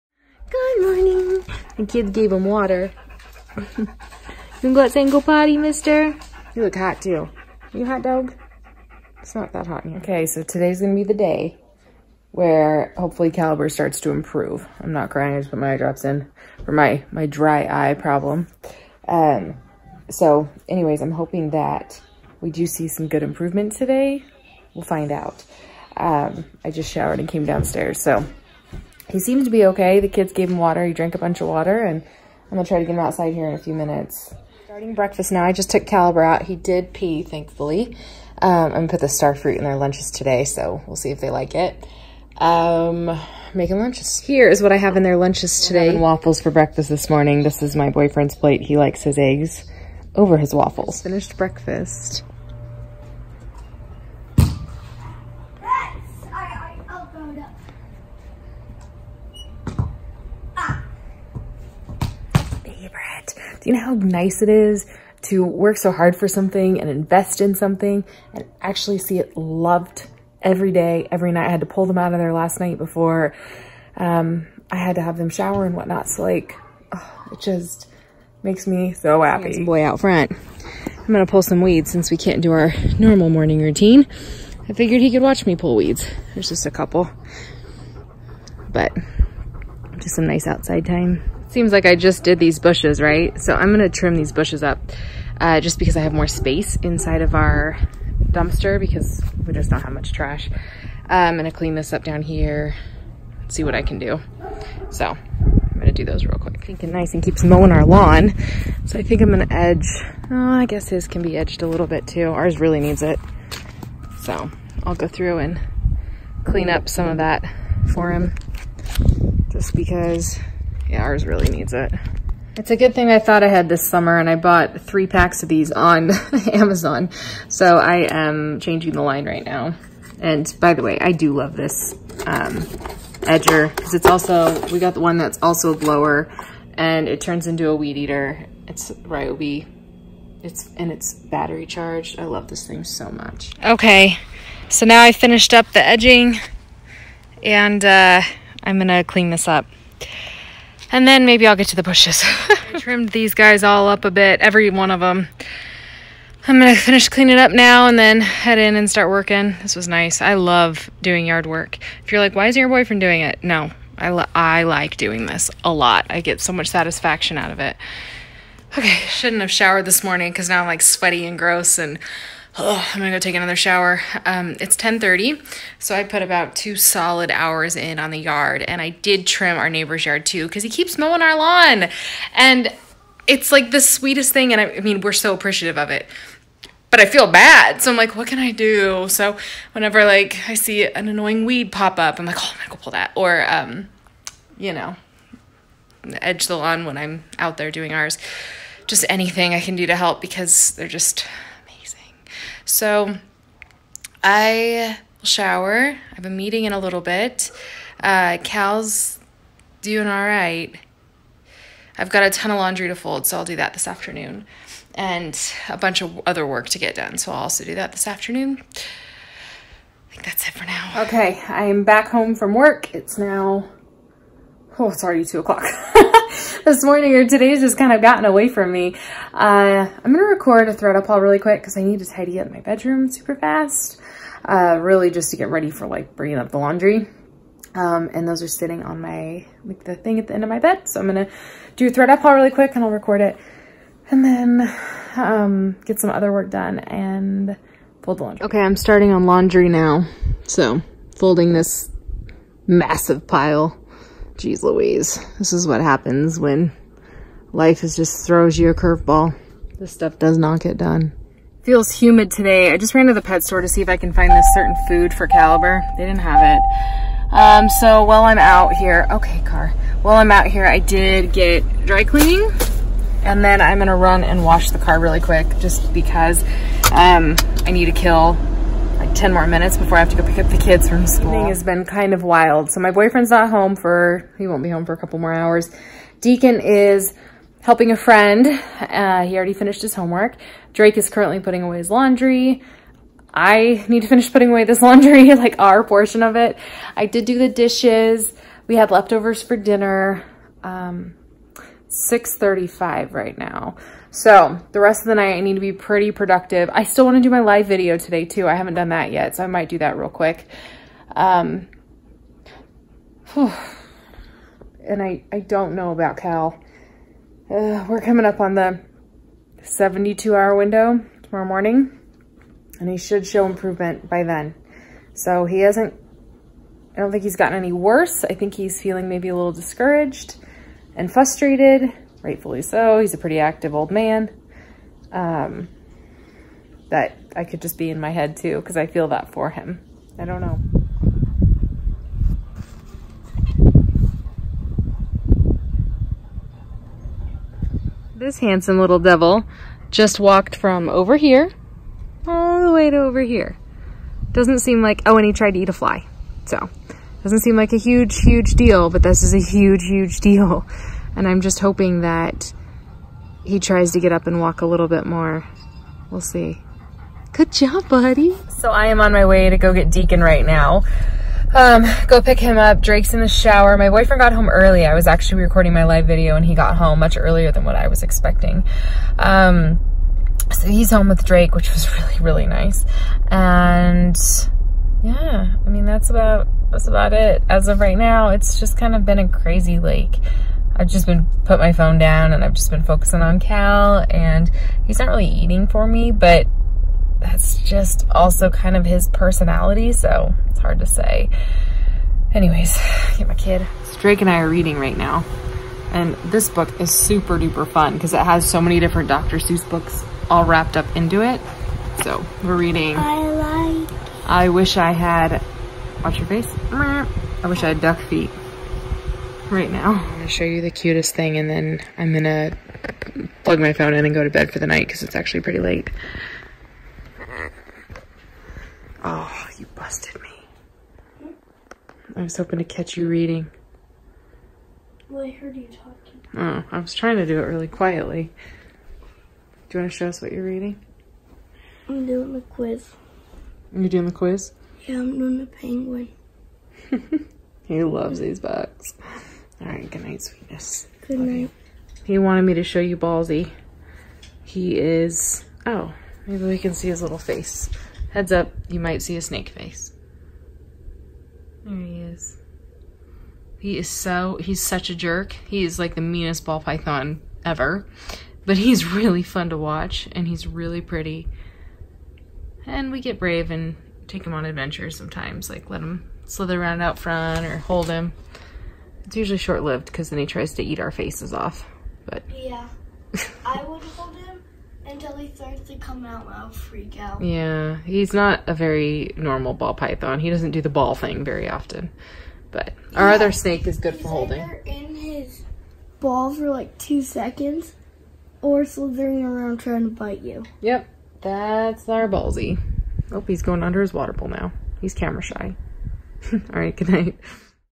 DITL | 8.22.19 | Day 3 Of Old Dog Vestibular Disease
This is a hard video to share... We are still unsure if he is going to pull through or not but in the meantime, this is what the disease/syndrome is.

Facebook.com/CandidMommy